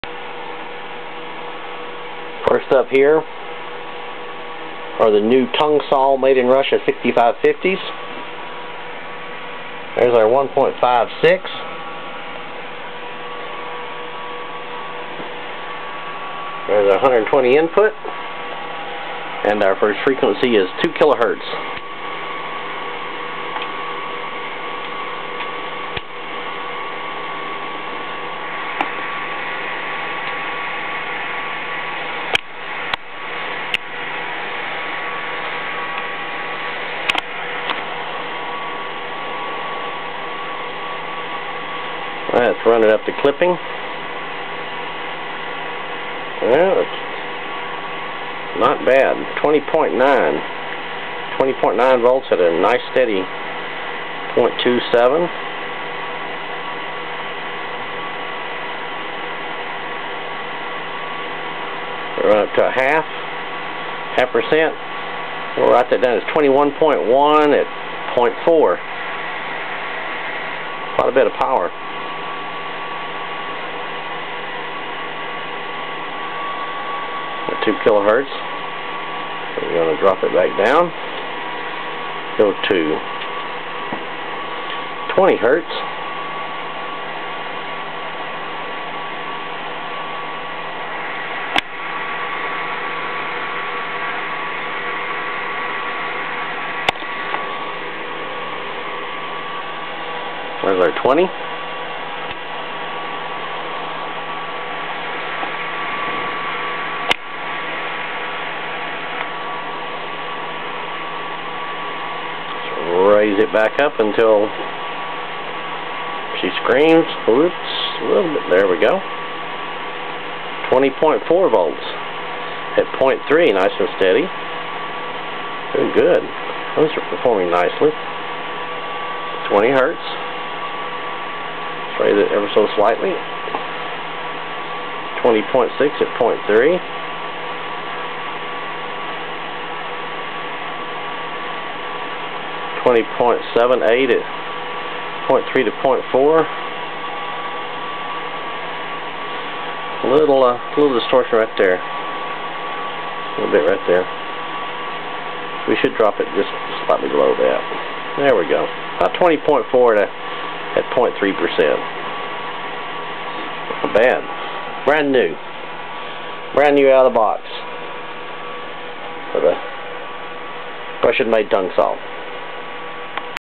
Modern Tung Sol 6550 Vacuum Tube.MOV
A comparison of the performance of new and vintage 6550's. The modern version of the Tung Sol 6550 is made in Russia